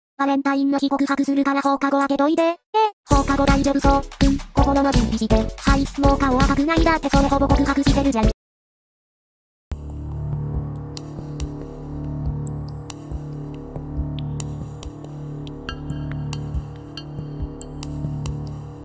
im gonna confess to you on valentines day!
ive been getting into horror-survival visual novels...cant let gang know i fw it

badger/ivar - creepy one with the buzzcut
deershk - ushanka guy

still doing my comic but im 2 lazy 2 link it[this video is just an au...]